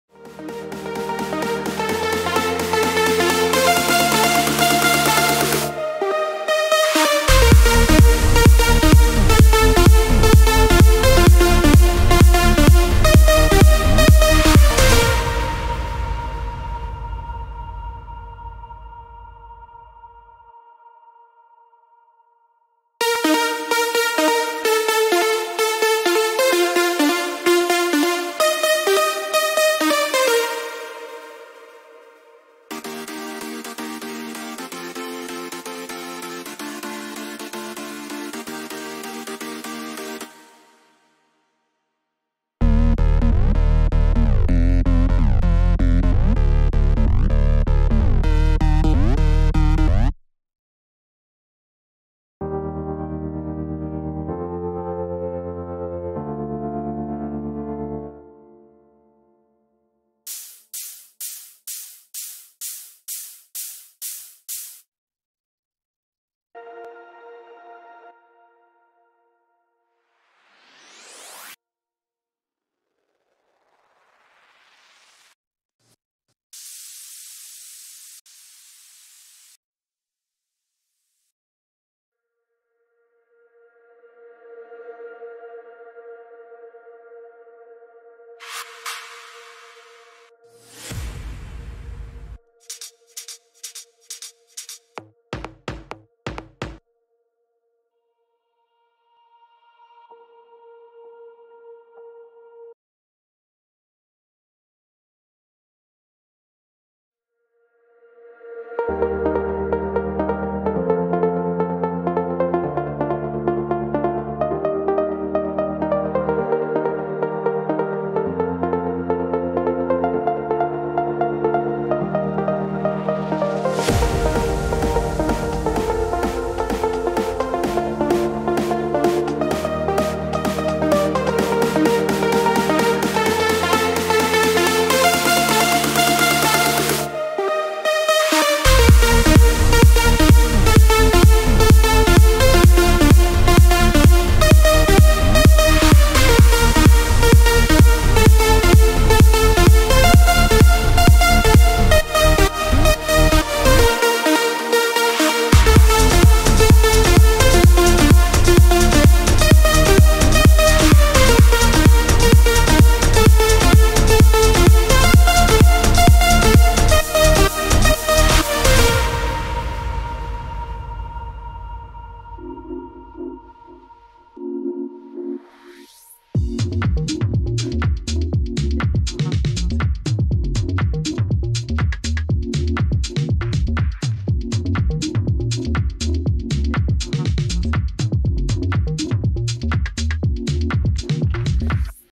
Professional Progressive House FLP / Template (Martin Garrix, Matisse & Sadko, Alesso, Manse)
VST's needed
- Sylenth1 v2.2
- Kickstart

This is a released song. Study the mixing, be inspired by the arrangement and take use of these techniques to help your future productions.